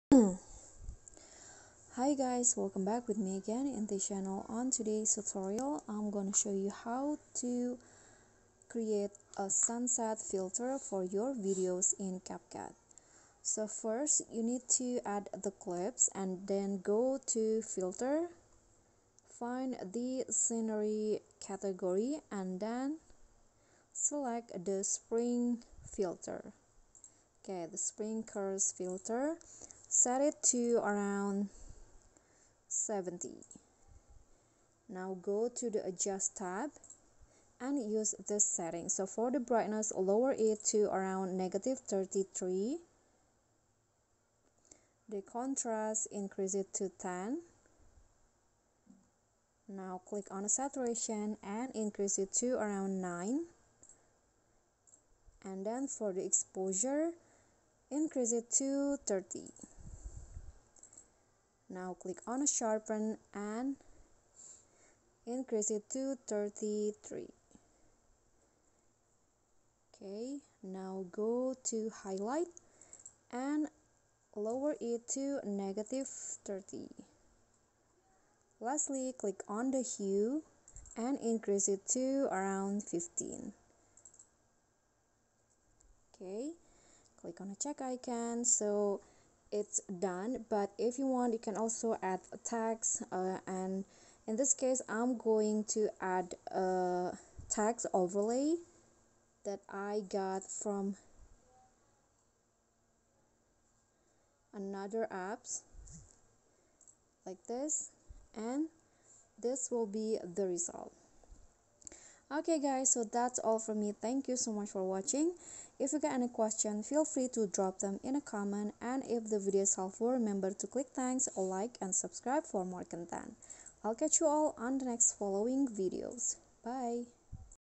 Sunset Filter Tutorial on CapCut! How to Create Sunset Filter for Your Videos in CapCut?
Want to create this sunset filter on your clips? Follow the steps in my tutorial!